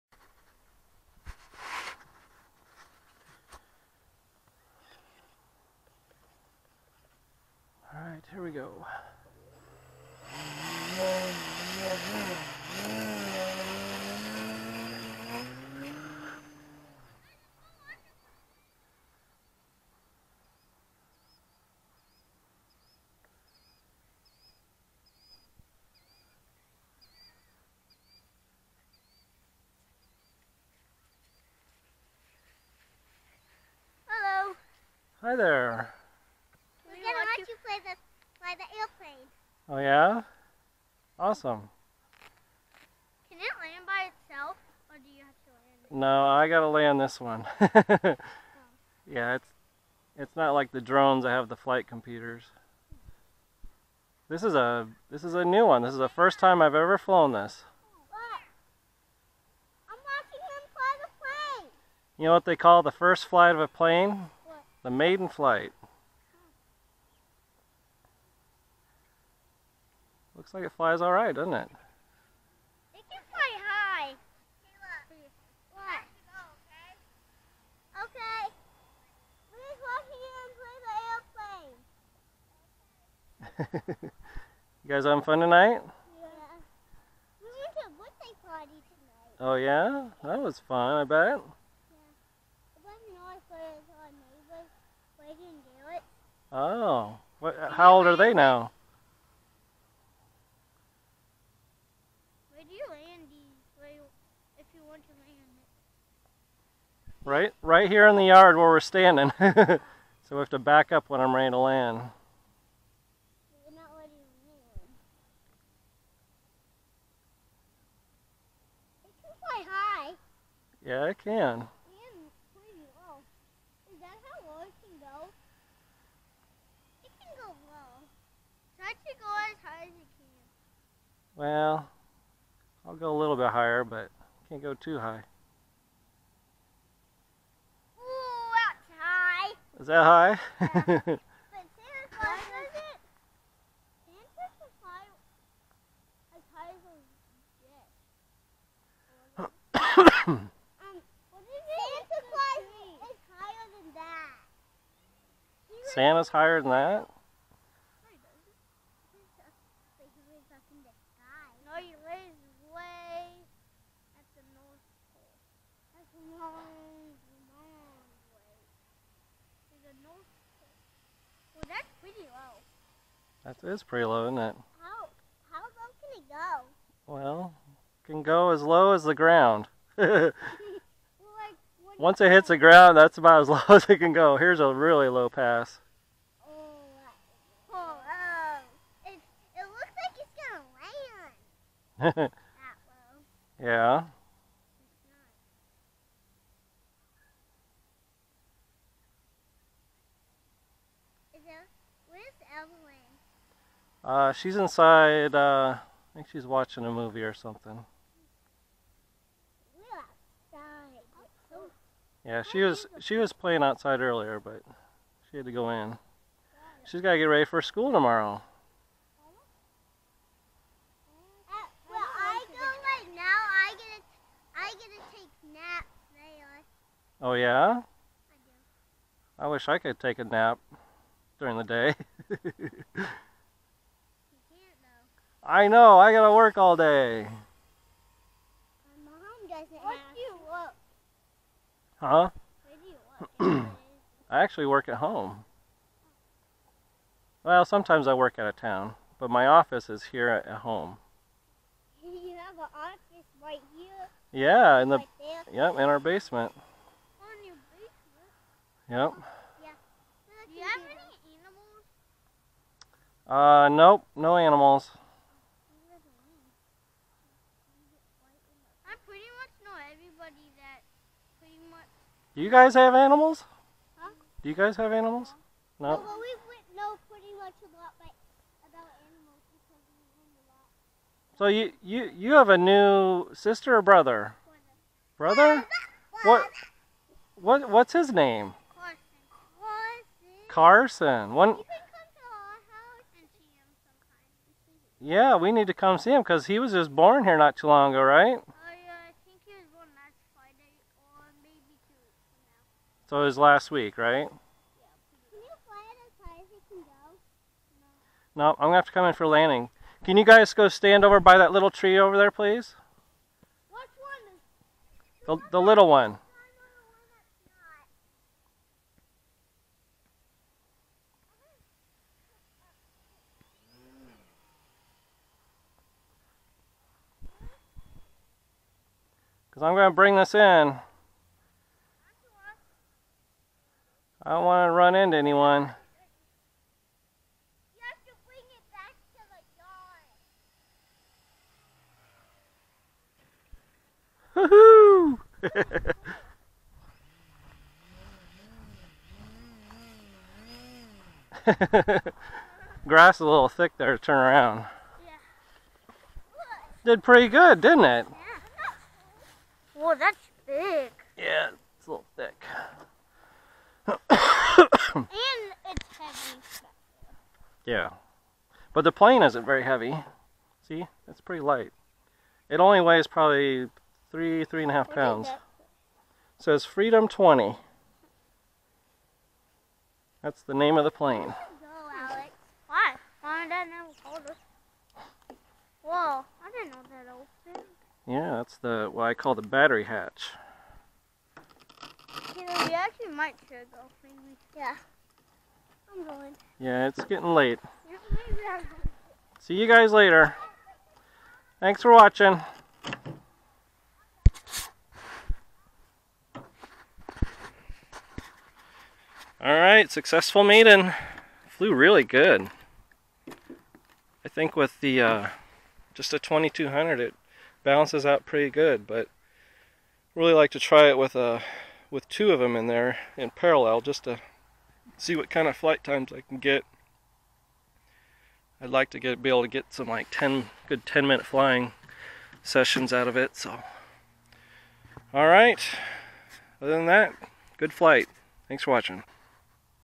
Carl Goldberg Freedom 20 EP Maiden Flight
This is the Freedom 20 balsa kit plane that I recently finished building. It was originally designed to be powered by a small Nitro/Glow engine. I converted to electric power using a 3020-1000kv brushless motor, 40A ESC, and  a standard 3s lithium polymer battery. It flies extremely well and is very relaxing to fly.